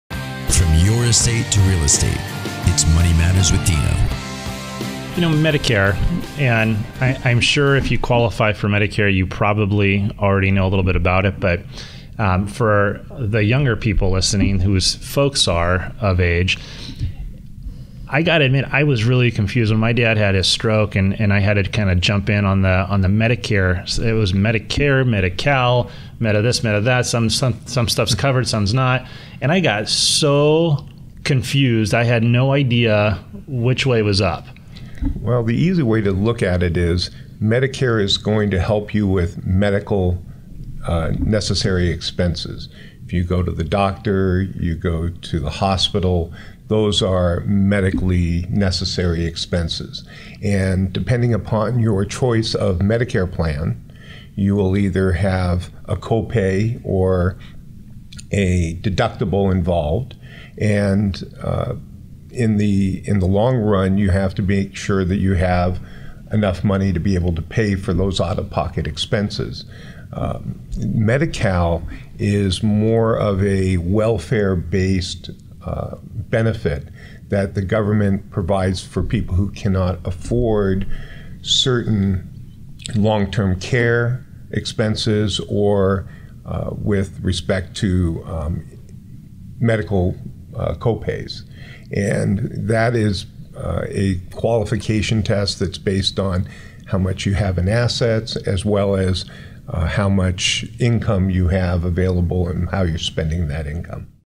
What Does Medicare Cover? - Michael Bader
Medicare is more than a little confusing, but we asked elder law attorney Michael Bader to explain it to us on our show "Retirement Goals and Senior Transitions"